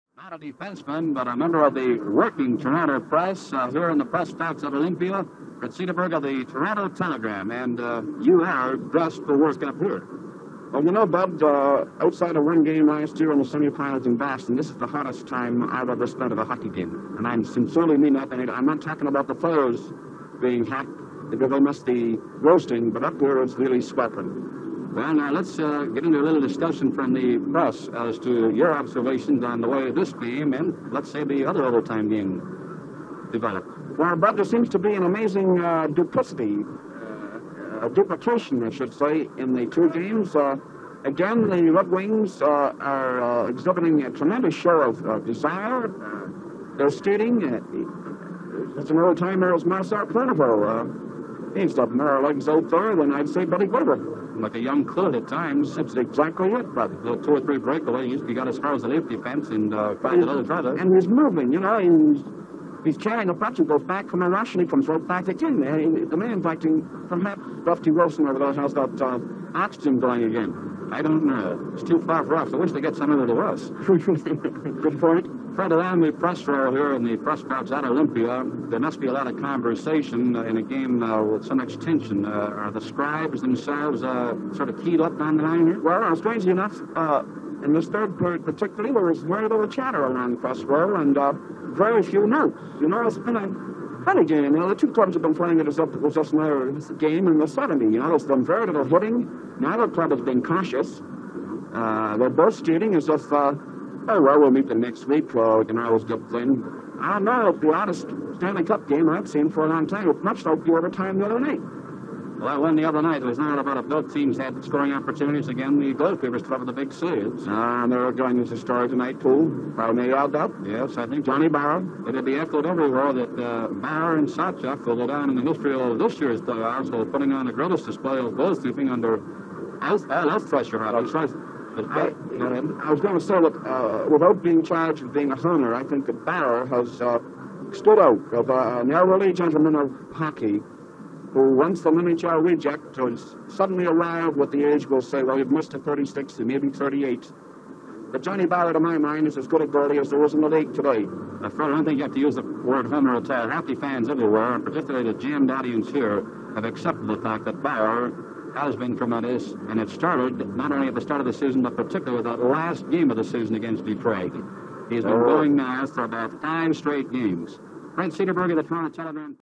Fred Cederberg 1960 April
Big Fred being interviewed during an intermission between the Toronto Maple Leafs and the Detroit Red Wings in April 1960